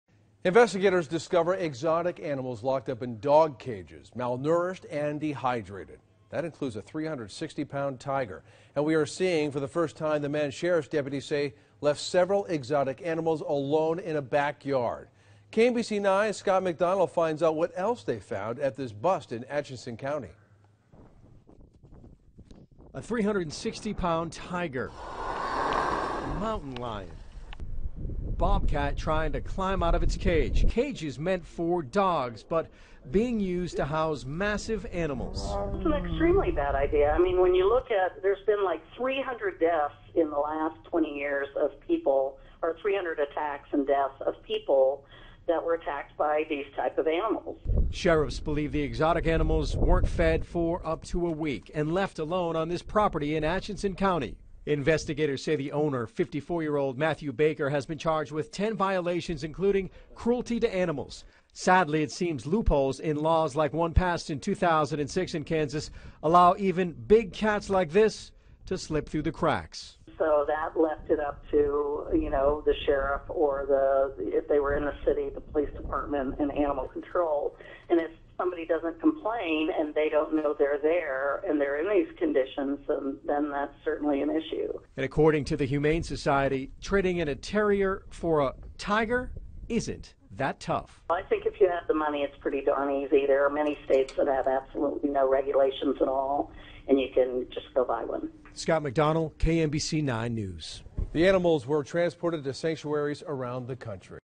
Tiger, exotic animals seized from Kansas home
Atchison County deputies said they seized a number of exotic animals, including a 350-pound tiger, from a man who had been keeping them in filthy and unsafe cages.